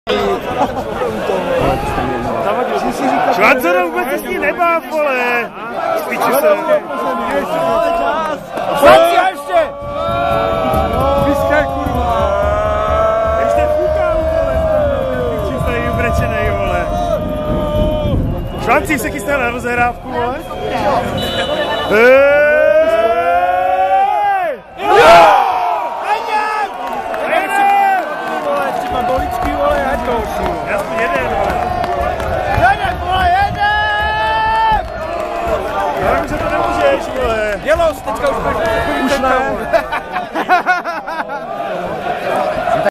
FC Zbrojovka Brno - FC Hradec Králové 1:3 (31.3.2013)
Proměněná penalta Pavla Zavadila na body nestačila :-(